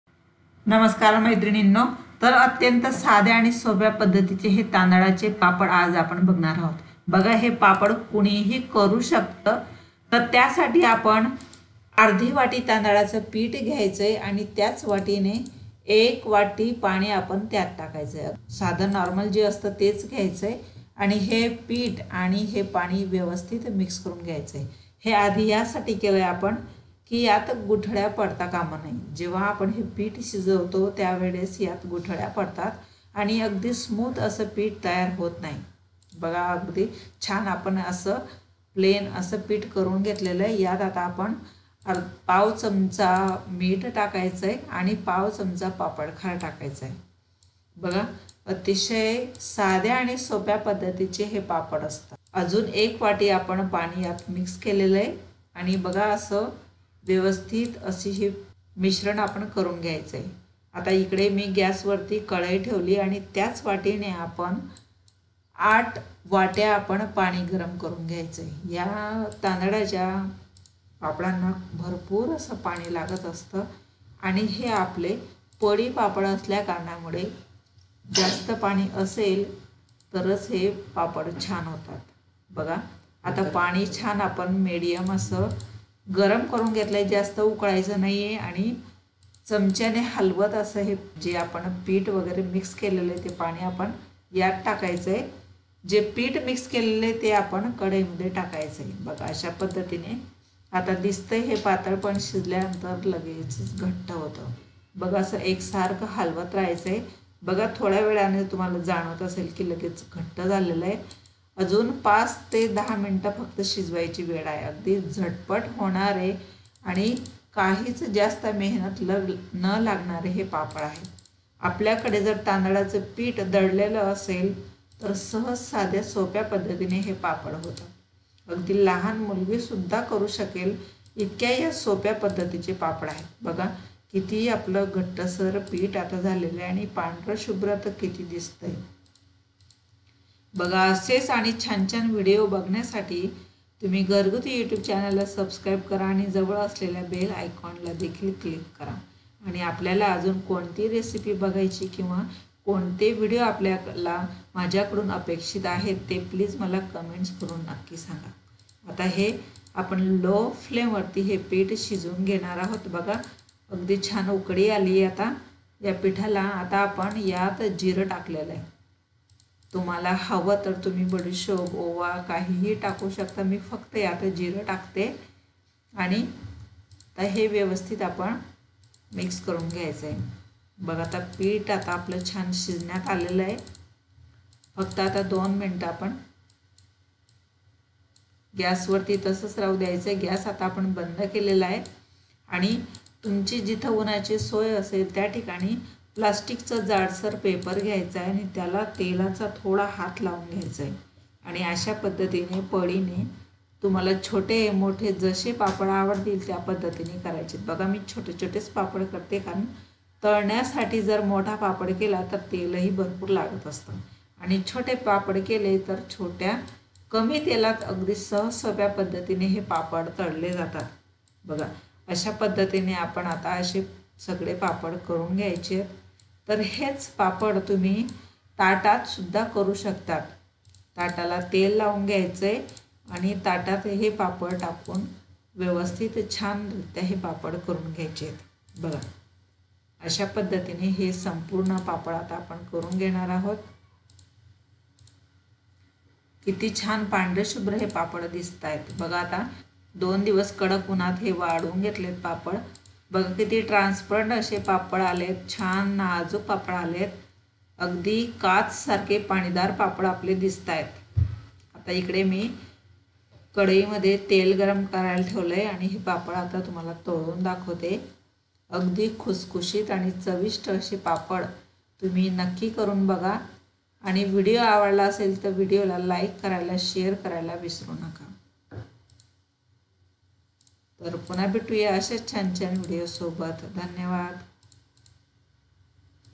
एक ट्रिक वापरून नक्की करा असे तांदळाचे पापड | पापडांची सर्वात सोपी पद्धत
कमी मेहनतीत आणि सहज सोपे होणारे तांदळाचे पापड आज आपण एका ट्रेक ने विकणे ट्रिक ने करून बघणार आहोत तर ही ट्रेक तुम्ही नक्की आपल्या उपयोगात आणा आणि अशा पद्धतीचे हे तांदळाचे पापड नक्की करून बघा आपल्याला जर पापड खावेसे वाटले आणि पारंपारिक पद्धतीने आपल्याला पापड जमत नसतील तर अशा पद्धतीचे साधे आणि सोपे पापड कोणीही करू शकेल अशा पद्धतीचे हे पापड आहेत या पापड करताना आपल्याला काही मेहनत करावी लागत नाही आणि काही आधीची तयारी सुद्धा करावी लागत नाही अगदी लगेचच हे पापड होतात आणि पारंपारिक पद्धतीने केल्यासारखेच चवीलाही आणि दिसायलाही तसेच पापड असतात तर मैत्रिणींनो अगदी कमीत कमी वेळेत आणि कमीत कमी साहित्य मध्ये हे पापड आपल्याला करता येतात तरीही पापडाची रेसिपी नक्की करून बघा आणि हे पापड कसे झाले हे कमेंट द्वारे कळवायला विसरू नका तसेच माझ्या व्हिडिओज आपल्या मित्र-मैत्रिणींना नातेवाईकांना शेअर करत जा म्हणजे त्यांनादेखील या रेसिपीज करता येतील तसेच आपल्याला अजून कोणता व्हिडिओ हवा आहे हे मला कमेंट द्वारे कळवायला विसरू नका धन्यवाद
तांदळाचे पापड कसे करावे पळी पापड कसे करावे घरगुती पापड रेसिपी